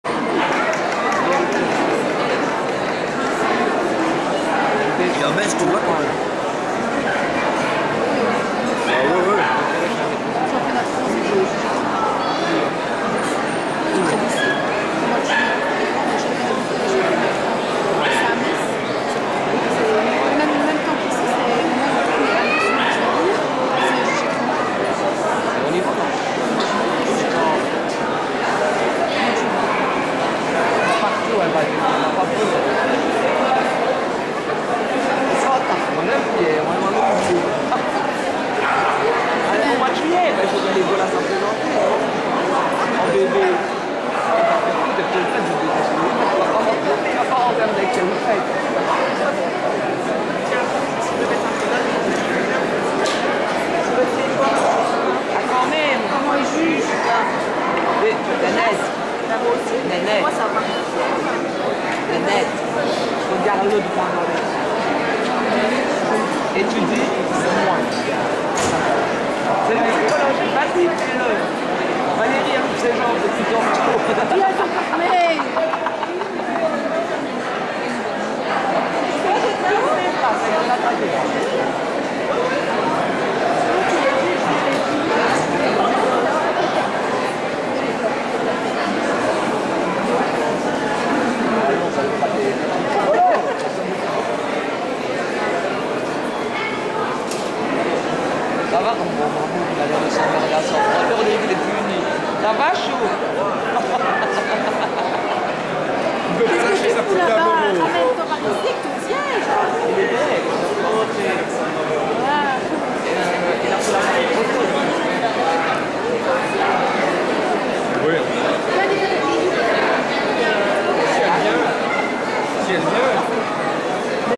International Dog Show Luxembourg April 1 2012 (part 2)
Indra getting the reserve CACIB after she got the CAC in Open class